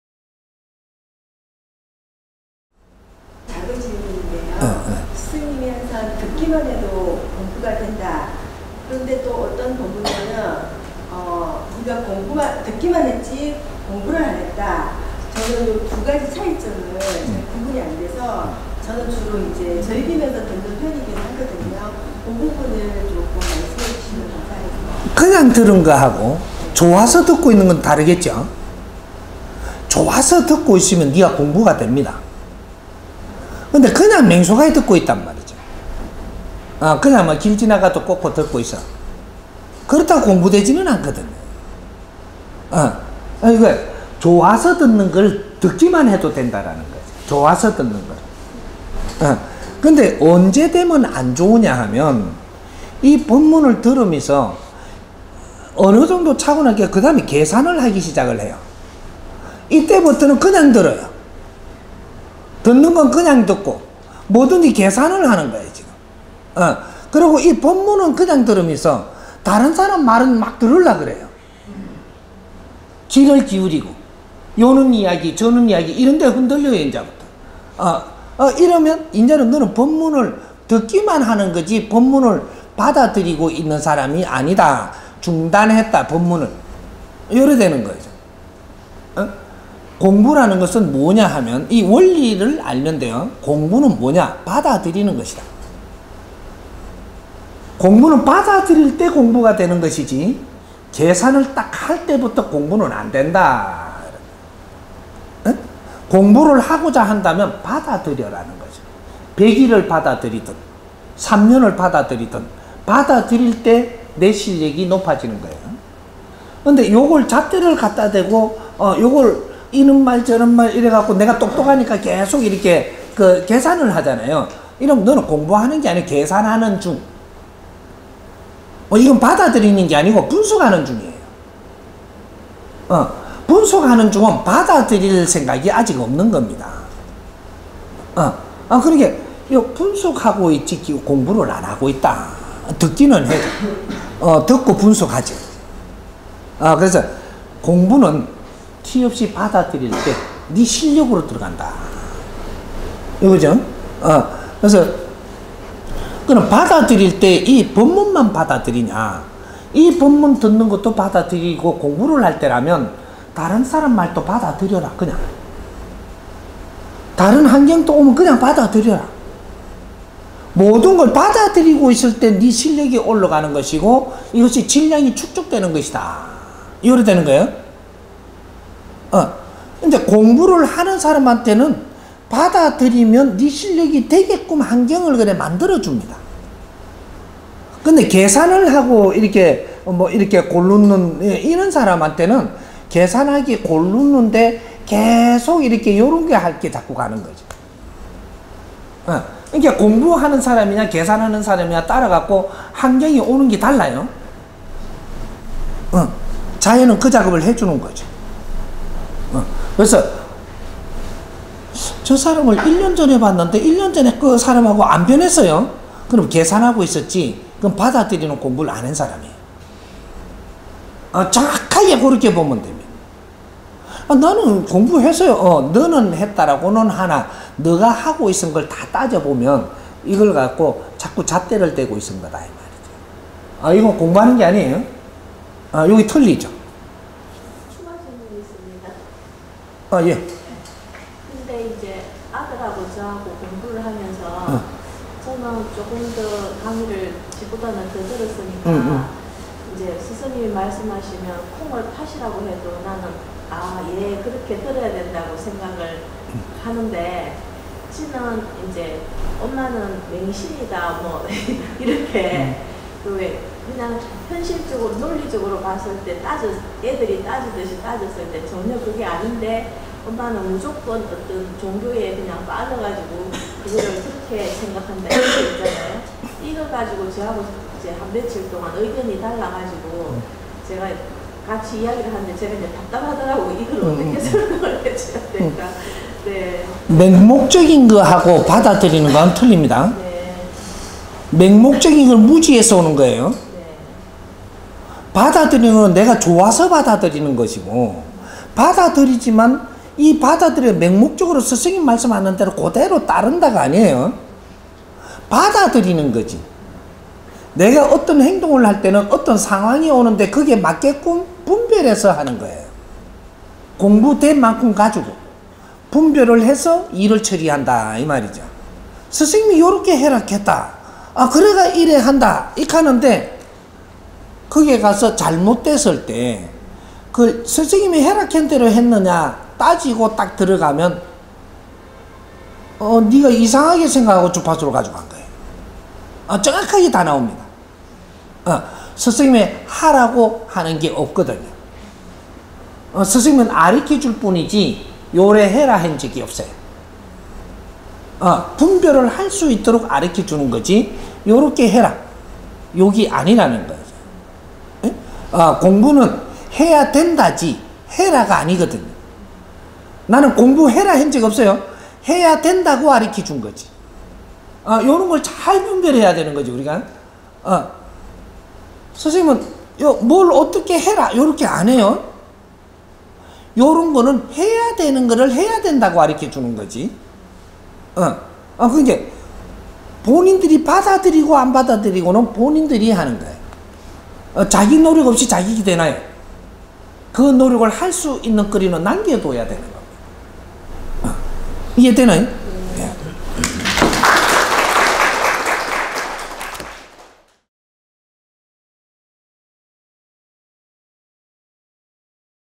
[홍익인간 인성교육] 7358강 듣기만 해도 공부가 된다 VS 듣기만 했지 공부를 안했다
질문 1 : 작은 질문입니다만, 스승님께서 "듣기만 해도 공부가 된다."고 하셨지만 또 다른 법문에는 "듣기만 했지, 공부를 안 했다."고 하셨습니다. 이 두 가지 차이점의 구분이 되지 않아서 (질문드립니다.)저는 주로 즐기면서 듣는 편인데 (바른) 공부법을 말씀해 주시기 바랍니다.
질문 2 : 추가 질문 있습니다. 아들과 함께 공부하고 있는데, 저는 아들보다 강의를 더 들었기 때문에 스승님께서 말씀하시면 콩을 팥이라 하셔도 그렇게 들어야 한다고 생각합니다. 그런데 아들은 "엄마는 맹신이다."라고 말합니다. 아들의 눈으로 봤을 때 현실적이거나 논리적으로 따져 보았을 때 전혀 아니라고 생각하는데 "엄마는 무조건 종교에 빠져서 생각한다."는 주장으로 저와 의견이 달라서 며칠간 이야기를 나누면서 답답했습니다. 그래서 어떻게 설득해 주어야 할지 궁금합니다.

강의 일자 : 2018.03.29 시흥 / 정법시대 : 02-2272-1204